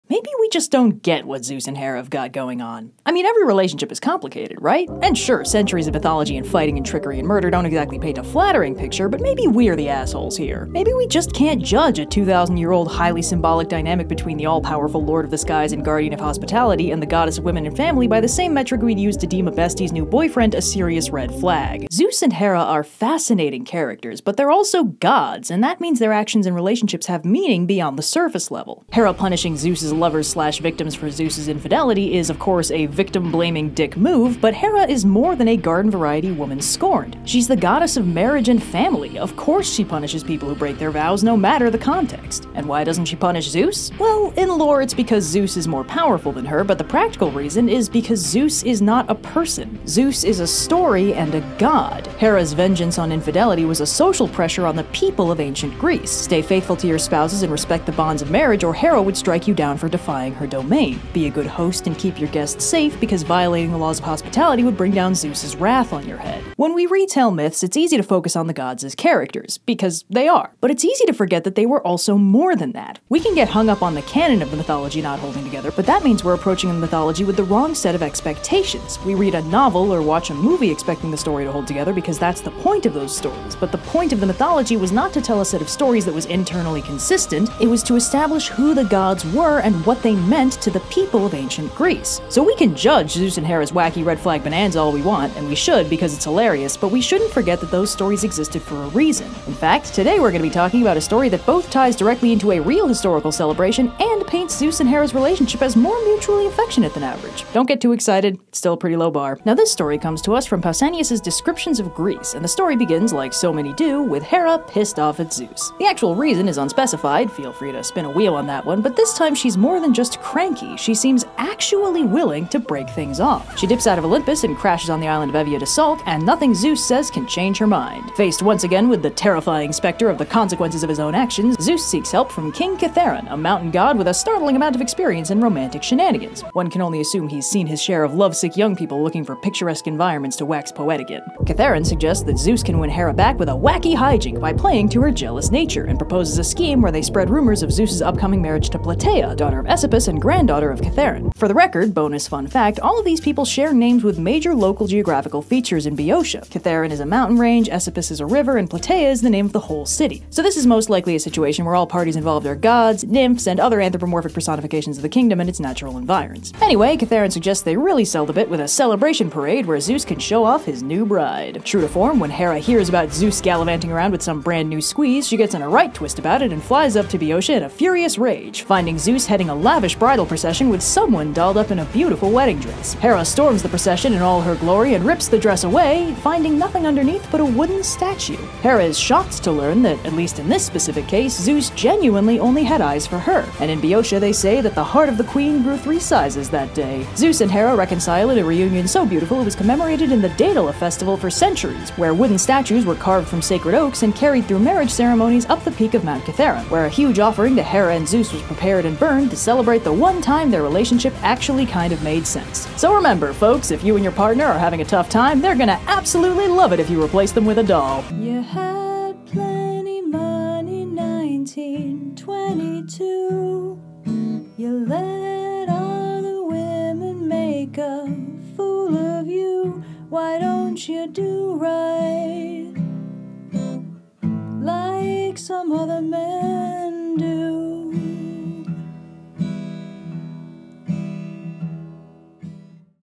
Miscellaneous Myths: Hera Crashes Zeus's Wedding
I'm sure there's a completely logical explanation for all of this. Don't worry, babe, he can explain!

Our content is intended for teenage audiences and up.